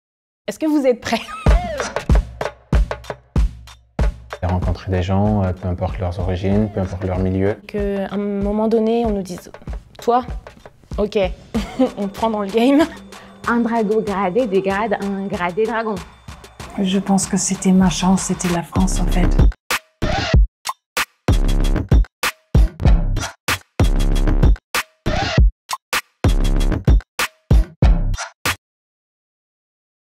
מה לגביך? כמה רחוק תגיע עם ידע בצרפתית ?
Et toi, jusqu’où iras-tu avec la langue française ?
Tu t’en rendras vite compte : tu vas pouvoir aller plusloin grâce à l'Institut français d’Israël !